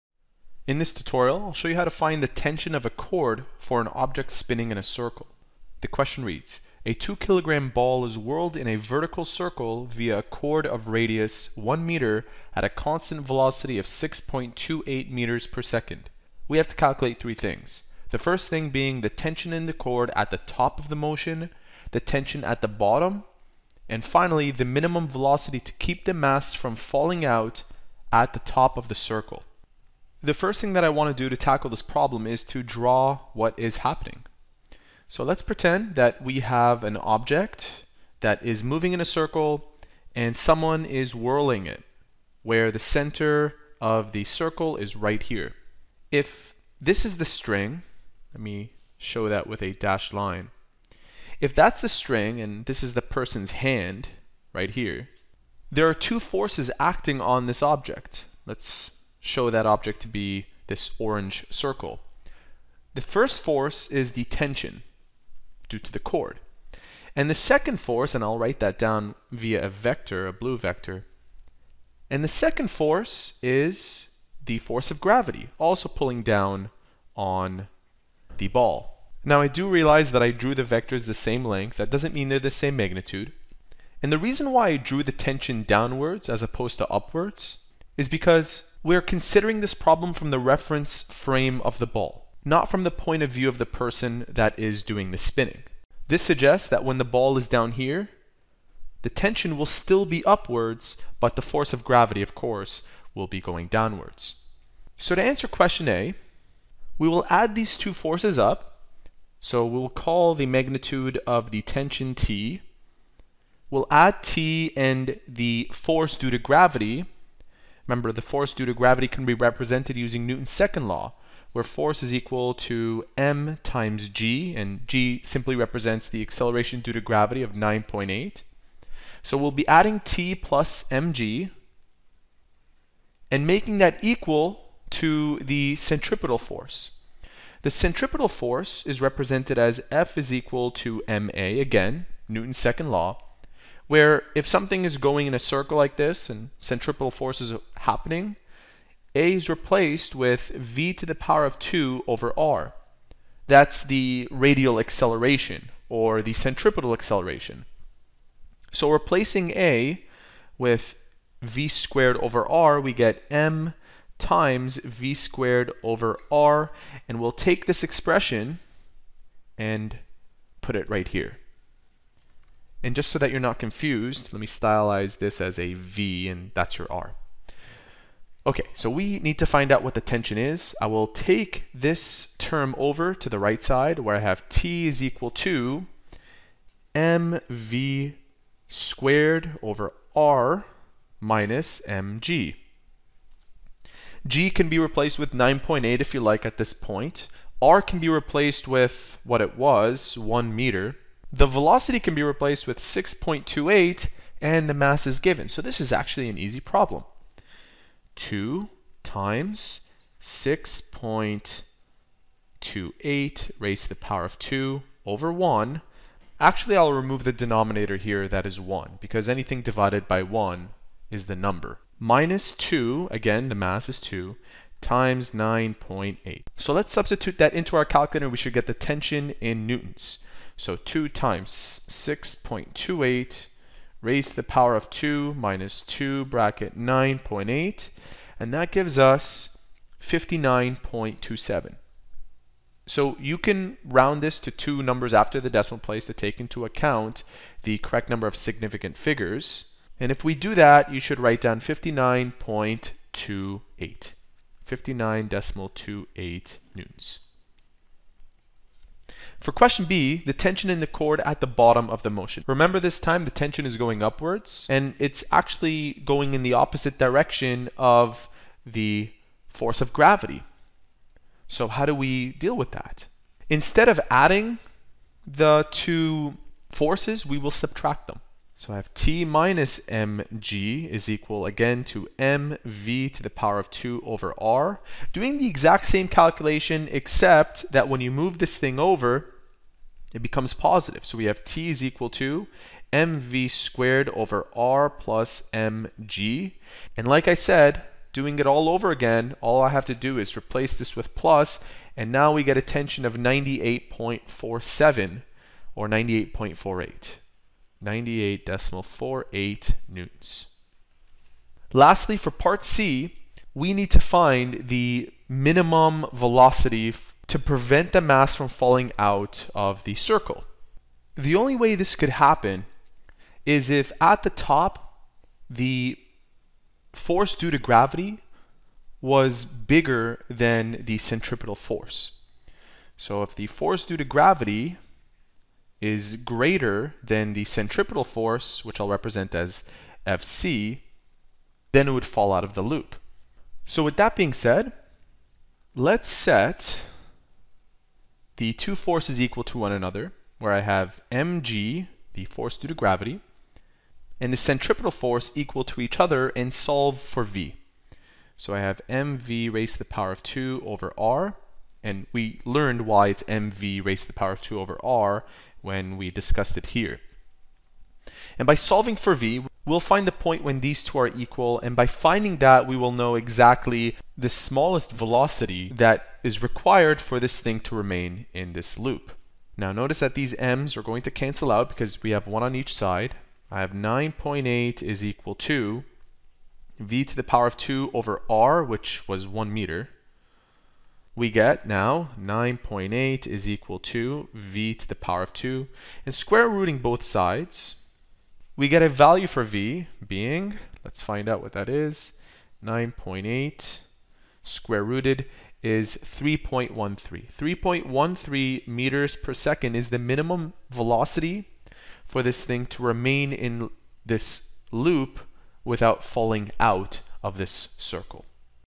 Calculate the Tension of a Cord for an Object Spinning in a Circle
A 2 kg ball is whirled in a vertical circle via a cord of radius 1 m at a constant velocity of 6.28 m/s. Calculate:

(a) the tension in the cord at the top of the motion,
(b) the tension in the cord at the bottom of the motion, and
(c) the minimum velocity to keep the mass from “falling out” at the top of the circle.